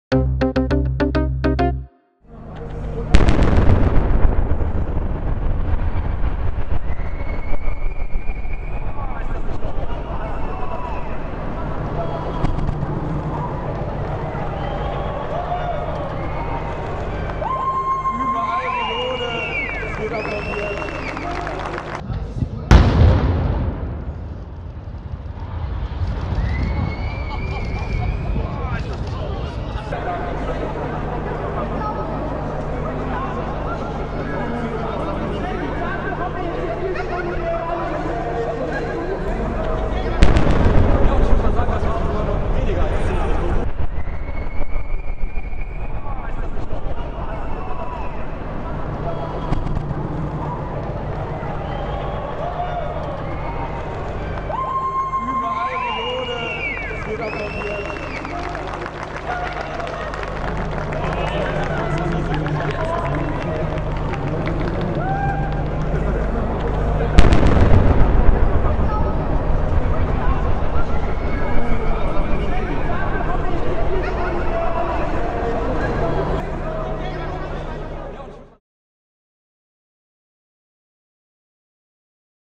Hochhaus „Weißer Riese“ in die Luft gejagt | Sprengung in Duisburg
Am Sonntag um Punkt 12 Uhr wurden 290 Kilogramm Plastiksprengstoff gezündet : Der „Weiße Riese“ mit seinen 320 Wohnungen auf 22 Stockwerken verwandelte sich in wenig Sekunden in 48 000 Tonnen Schutt!